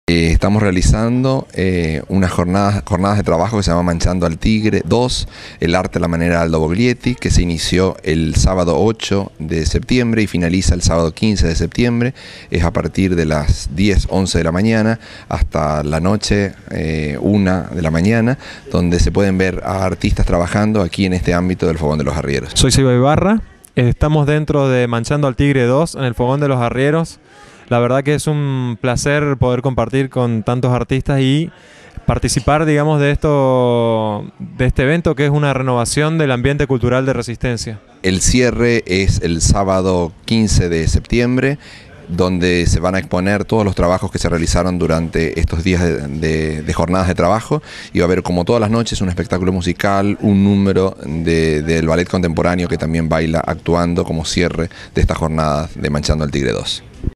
Manchando al Tigre 2
Manchando al Tigre 2: en la entrevista: Martín Gustin y Seba Ibarra.
info: Desde el 8 y hasta el 15 de septiembre el Fogón de los Arrieros de Resistencia vive un nuevo encuentro para disfrutar del espacio cultural junto a artistas locales y apreciar sus creaciones, comidas y buena música en vivo.